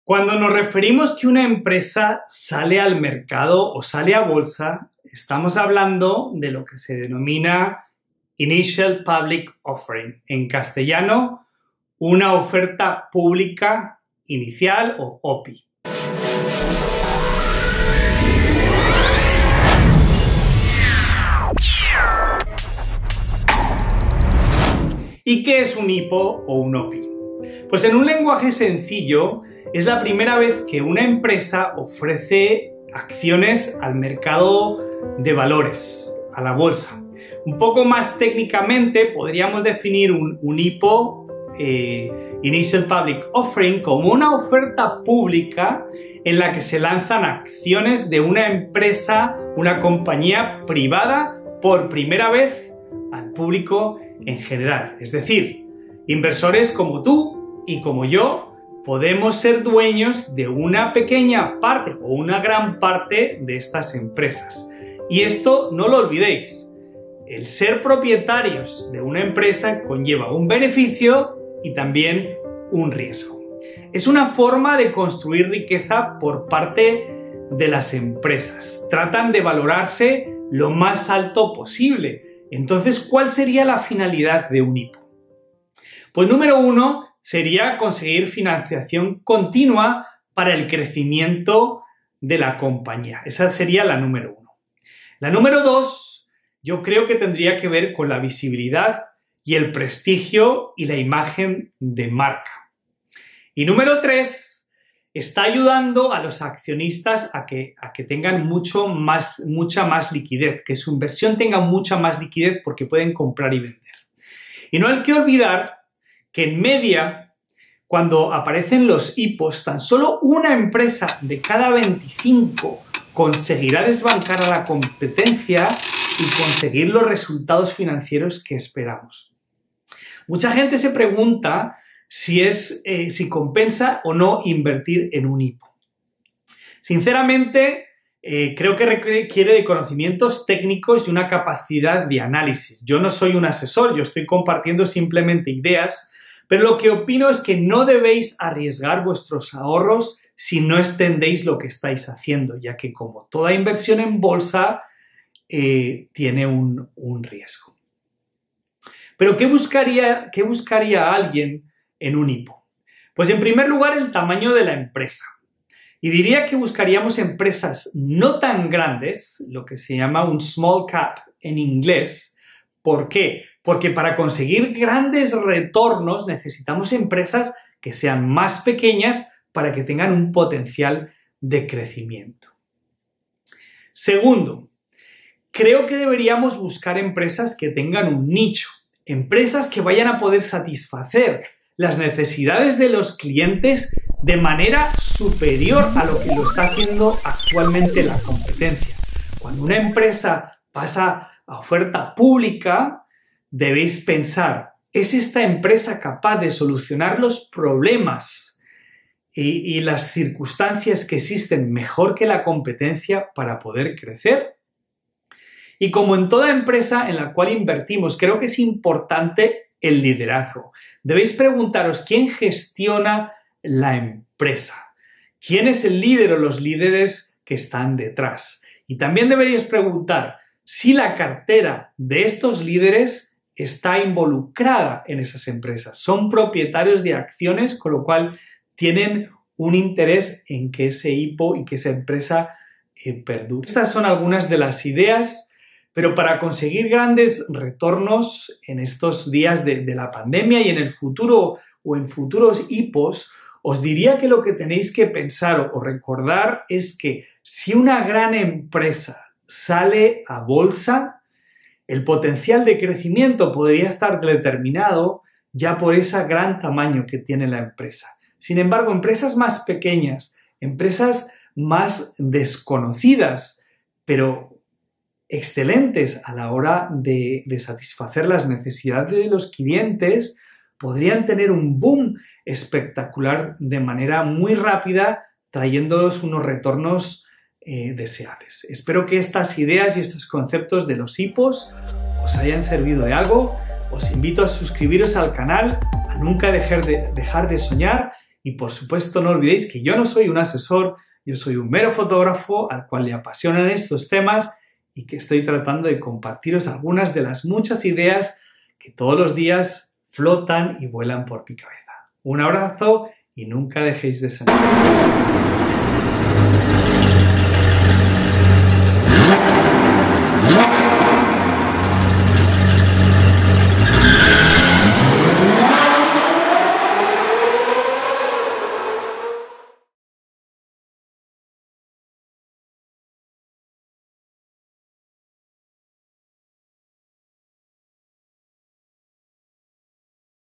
💲 APRENDE AHORA 💲 ¿Qué es una IPO y una OPI? 🚀 FINANZAS para (Principiantes) + (Emprendedores)
¿Qué es una IPO y una OPI? 🚀 Finanzas para Principiantes : En este episodio explico qué es una IPO (Initial Public Offering) también conocida en castellano como OPI (Oferta Pública Inicial). Hablo de las razonas por las que una empresa decide salir al mercado de valores, como por ejemplo capital para poder crecer, prestigio y credibilidad y mayor liquidez para las inversiones de sus accionistas.
1 de cada 25 IPOs obtienen los resultados esperados y consiguen sobresalir sobre la competencia. Es por ello que hago algunas reflexiones sobre qué debe de buscar un inversor para obtener grandes rendimientos en su inversión. Debe valorar el liderazgo de la empresa, el tamaño actual, y el nicho en el que se desempeña, entre otras cosas

► Prueba Audible y recibe dos audiolibros gratis
► Explore Colombia finanzas para emprendedores que es una opi